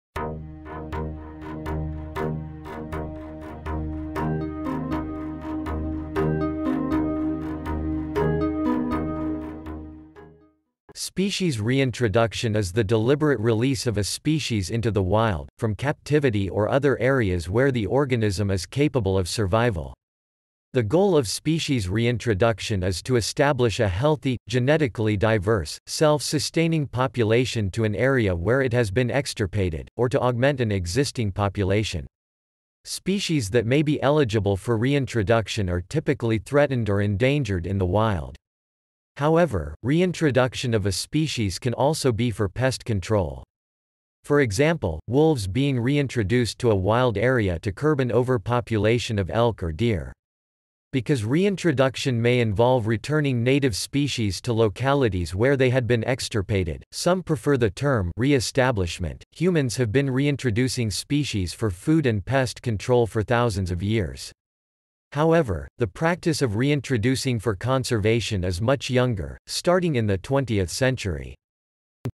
What is Species reintroduction?, Explain Species reintroduction, Define Species reintroduction
~~~ Species reintroduction ~~~

Title: What is Species reintroduction?, Explain Species reintroduction, Define Species reintroduction

Description: Species reintroduction is the deliberate release of a species into the wild, from captivity or other areas where the organism is capable of survival. The goal of species reintroduction is to establish a healthy, genetically diverse, self-sustaining population to an area where it has been extirpated, or to augment an existing population. Species that may be eligible for reintroduction are typically threatened or endangered in the wild. However, reintroduction of a species can also be for pest control. For example, wolves being reintroduced to a wild area to curb an overpopulation of elk or deer. Because reintroduction may involve returning native species to localities where they had been extirpated, some prefer the term "reestablishment".Humans have been reintroducing species for food and pest control for thousands of years. However, the practice of reintroducing for conservation is much younger, starting in the 20th century.